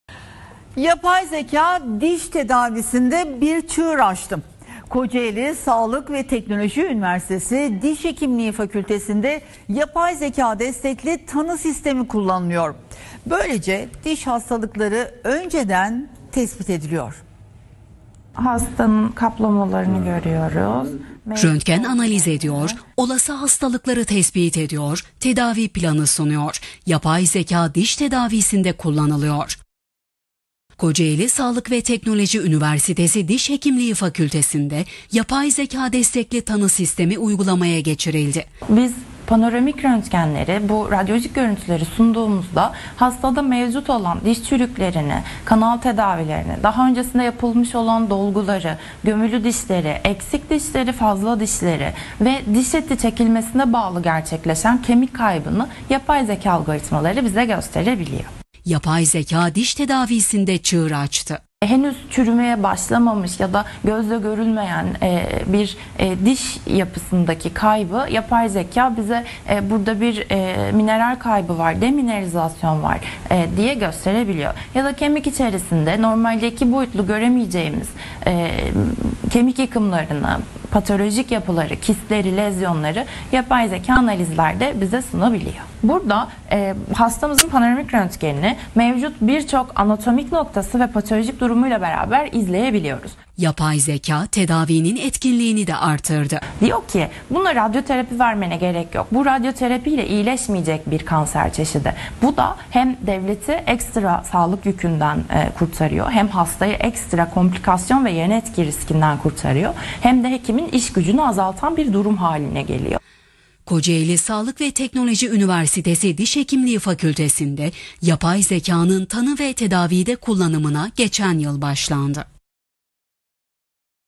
Diş Hekimliğinde Yapay Zeka Teknolojisi | TRT Haber
Yapay zeka, Türkiye’deki üniversitelerde diş hekimliği eğitimi ve klinik uygulamaları dönüştürüyor. Bu TRT Haber özel haberinde, CranioCatch’in Kocaeli Sağlık ve Teknoloji Üniversitesi’ndeki kullanımına ve yapay zekanın diş röntgeni analizinde nasıl fark yarattığına dair detayları bulabilirsiniz.

Birçok üniversite, tanısal doğruluğu artırmak, iş akışlarını kolaylaştırmak ve öğrenci eğitimini geliştirmek için CranioCatch gibi yapay zeka destekli çözümleri benimsemeye başladı. Bu yenilikçi teknolojinin diş hekimliğinin geleceğini nasıl şekillendirdiğini keşfedin.